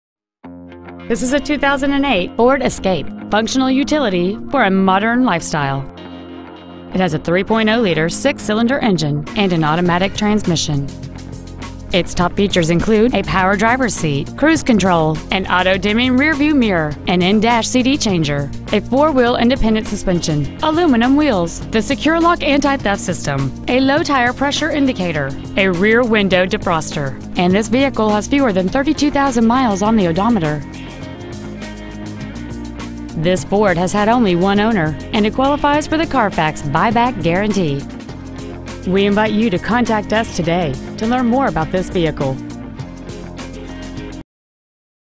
2008 Ford Escape Sylvania OH
Year : 2008
 Make : Ford
 Model : Escape
 Engine : 3.0L DOHC SEFI 24-valve V6 Duratec engine
 Trans . : Automatic
 Exterior : Black
 Miles : 31,536
 Interior : Other

 2008 Ford Escape Sylvania OH